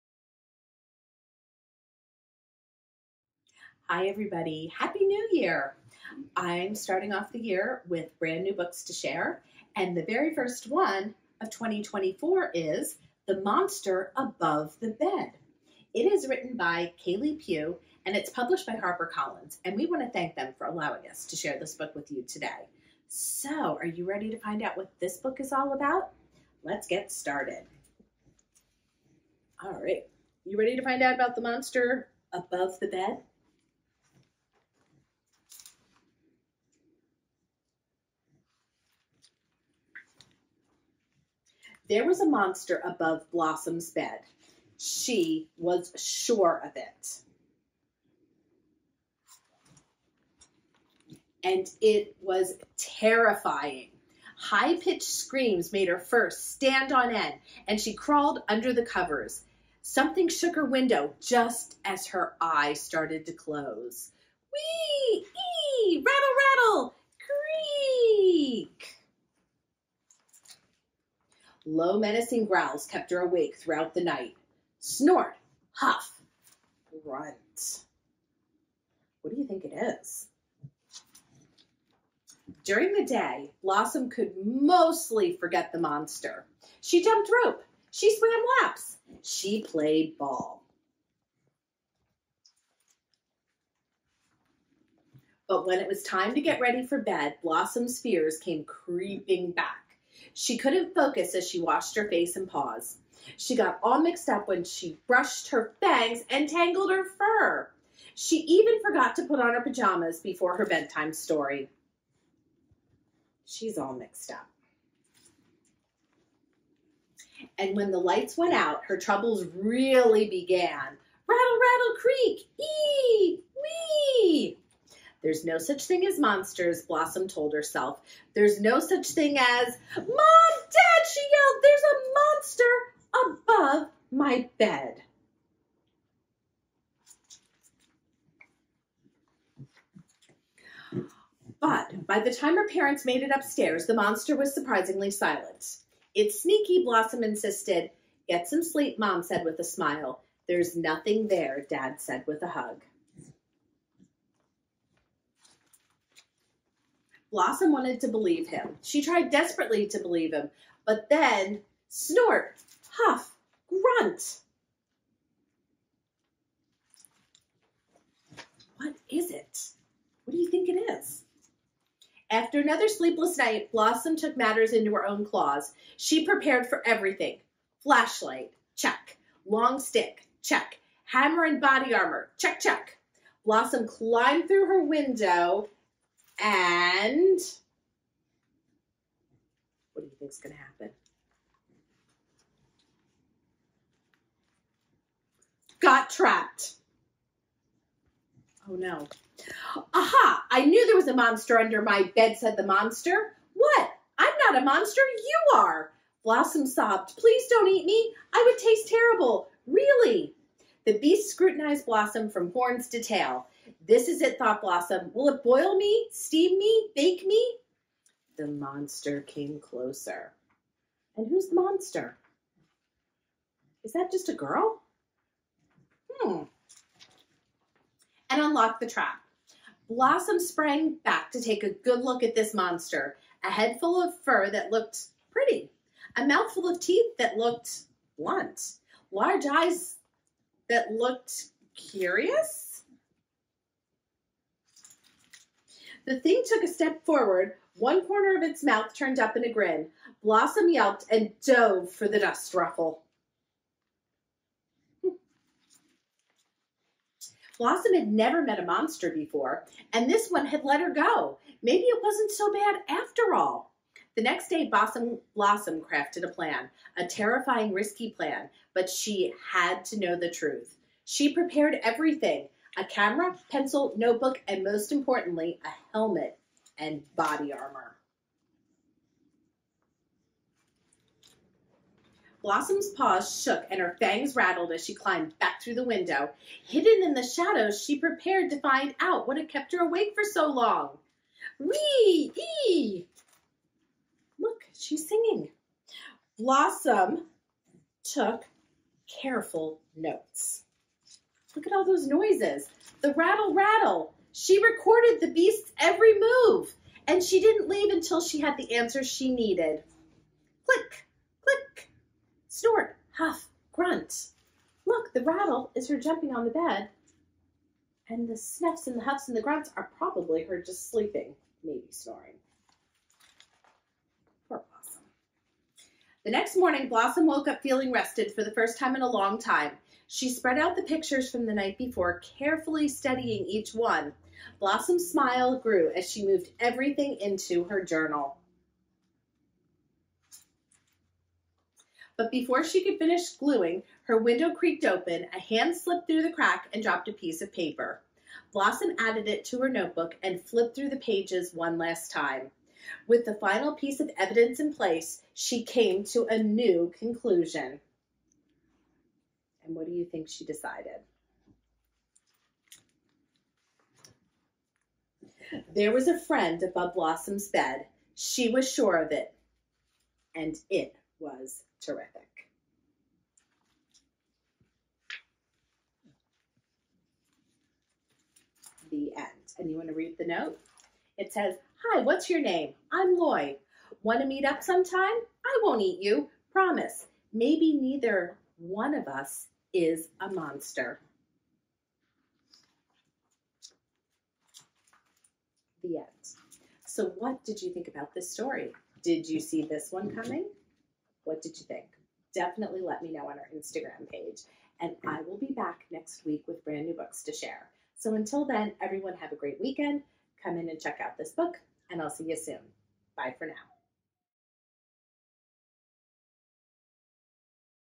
Story Time 314 - The Monster Above the Bed
Blossom loves jumping snake, swimming in the bog, and playing slime ball. She doesn’t love the creepy sounds above her bed that no one else seems to notice.
Determined to get to the top of it once and for all, Blossom hatches a plan that leads her to find courage…and a new friend.
This wholesome twist on a familiar fear that will make you wonder—what do the monsters under our beds think about us? Find out in "The Monster Above the Bed" by Kailei Pew.